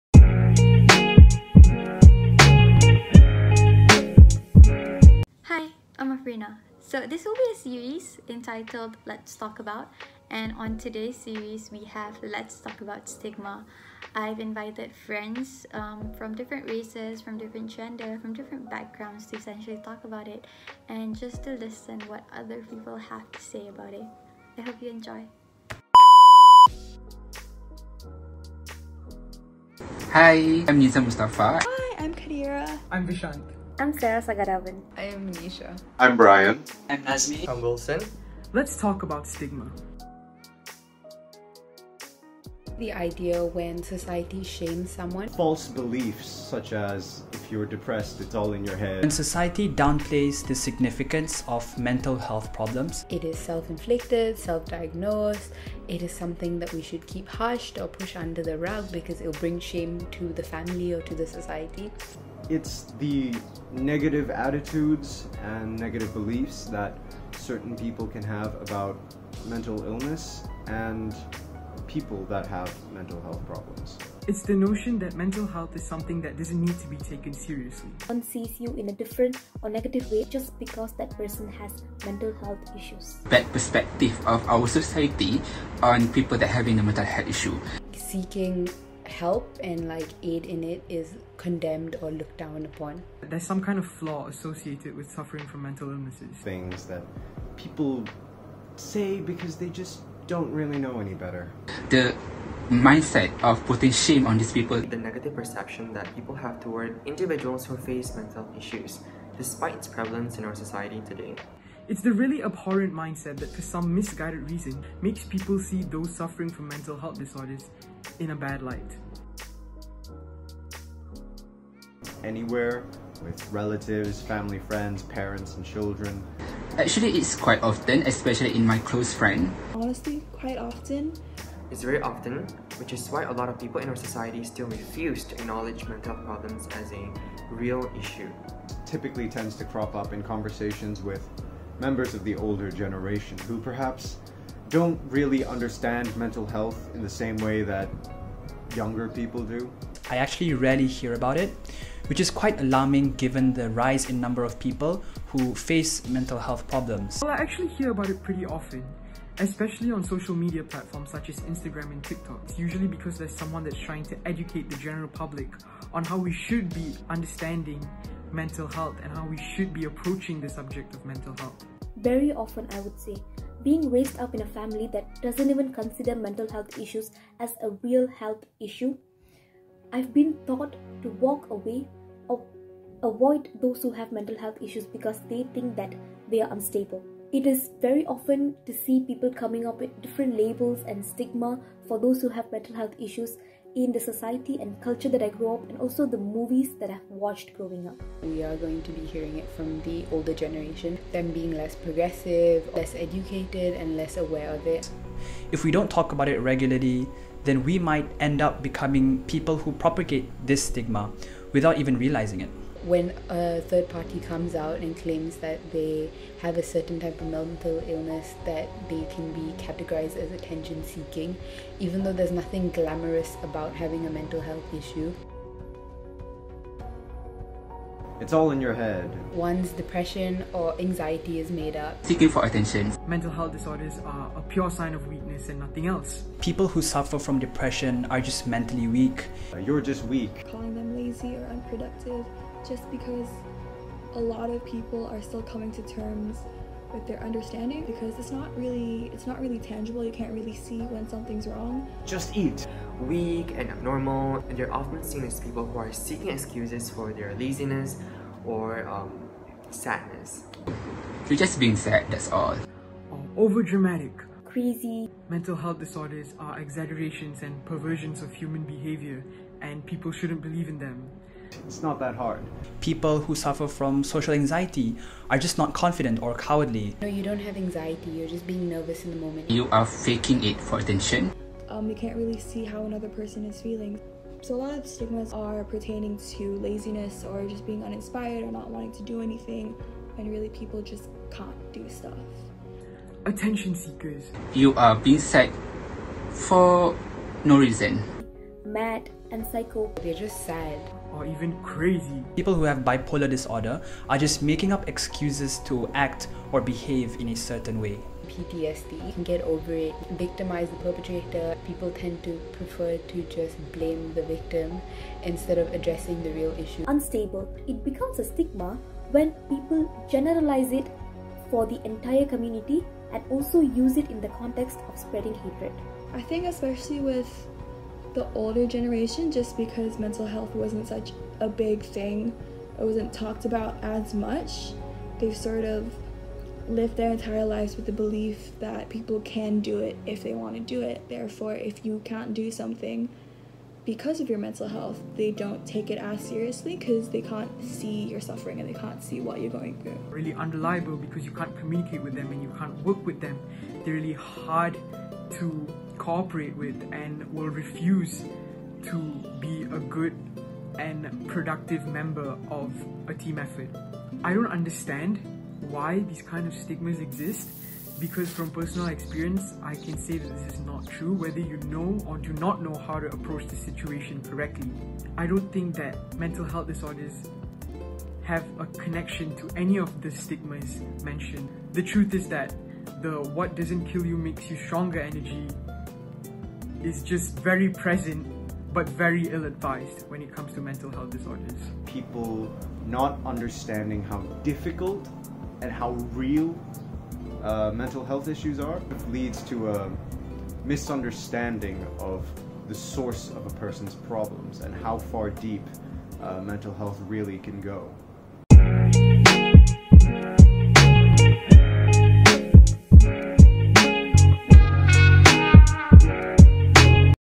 Episode 1 : Mental Health Stigma Part 1
This series called 'Let's Talk About' is inspired but how in our current society people are reluctant to talk about these topics be it mental health, mental illness, disorders and more! Instead of talking about it by myself, I decided to invite friends and acquaintances to give their personal take on these topics. Hoping to make it a weekly series, we are kicking off with the first topic "Let's Talk About Mental Health Stigma".